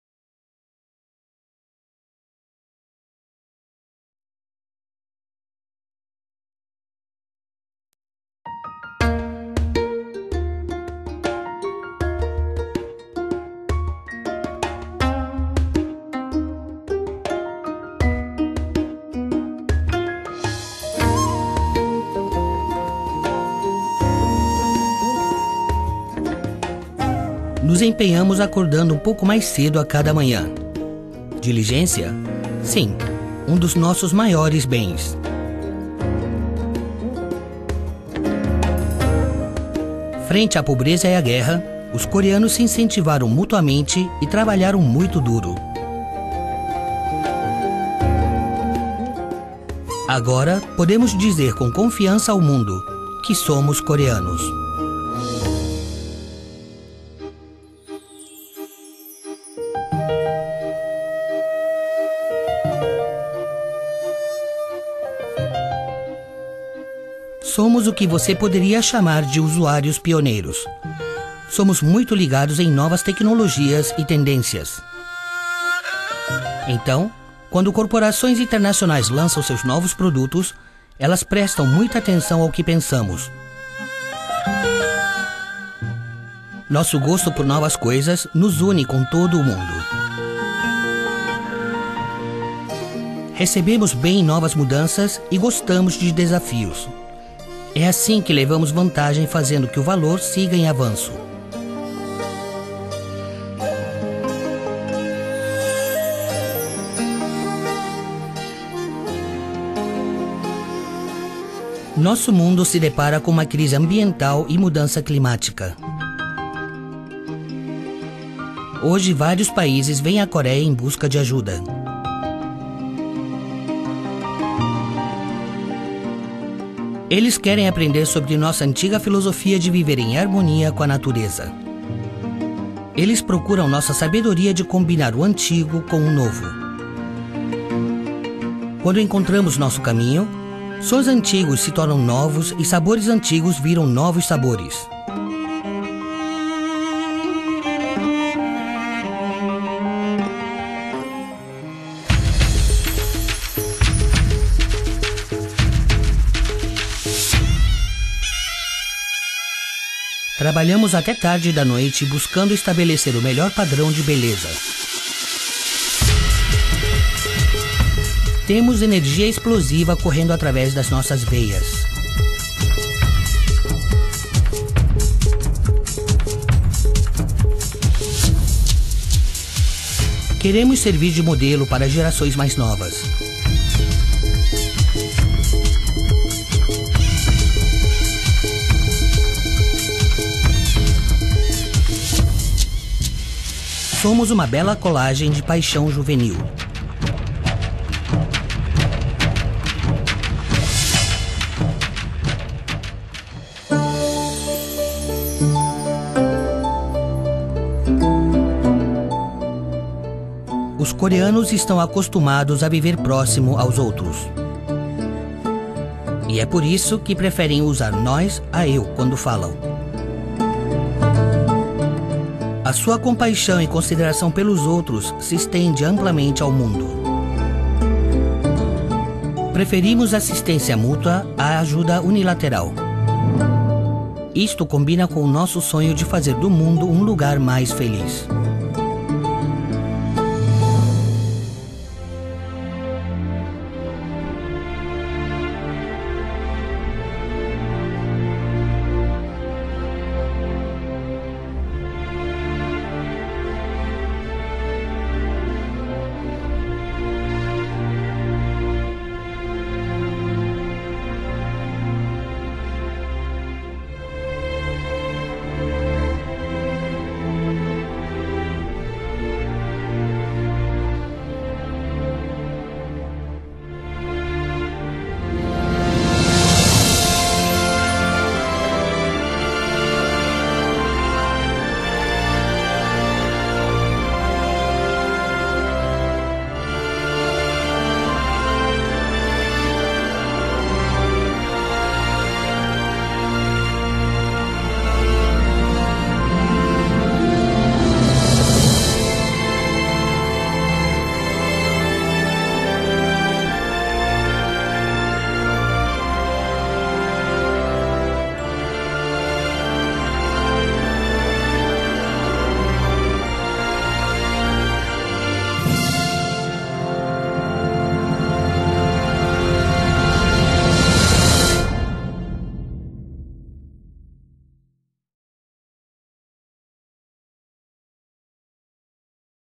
(Portuguese)Korea, Where dreams are realized every day
The video highlights various aspects of Korea, including the country's postwar economic development experience and Korean culture.